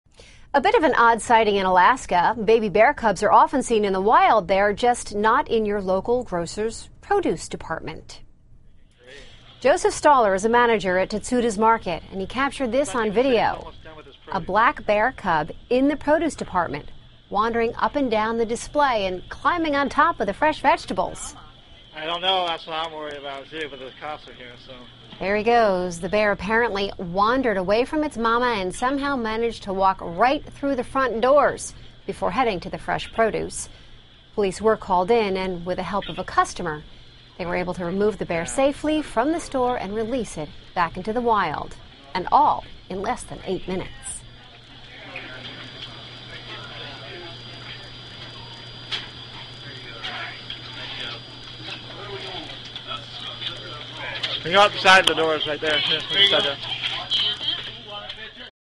a_tdy_brown_cub_111018
Black bear cub in Sitka supermarket. News story.